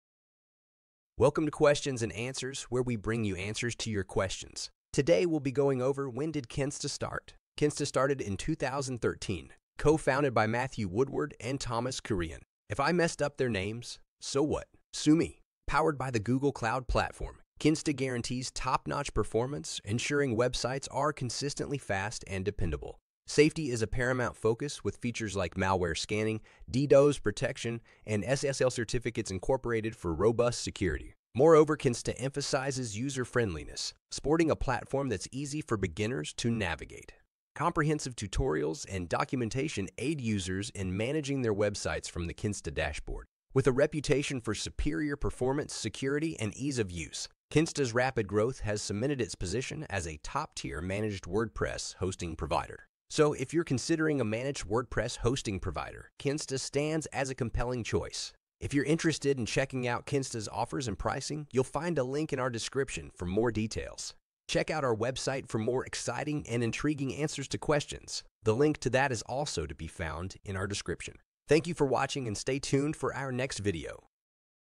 When Did Kinsta Start?
Kinsta, co-founded by Matthew Woodward and Thomas Kurian in 2013, stands out as a leading provider of managed WordPress hosting. Powered by Google Cloud Platform, Kinsta guarantees top-notch performance, ensuring websites are consistently fast and dependable. Safety is a paramount focus, with features like malware scanning, DDoS protection, and SSL certificates incorporated for robust security. Moreover, Kinsta emphasizes user-friendliness, sporting a platform that’s easy for beginners to navigate. Comprehensive tutorials and documentation aid users in managing their websites from the Kinsta dashboard. With a reputation for superior performance, security, and ease of use, Kinsta’s rapid growth has cemented its position as a top-tier managed WordPress hosting provider.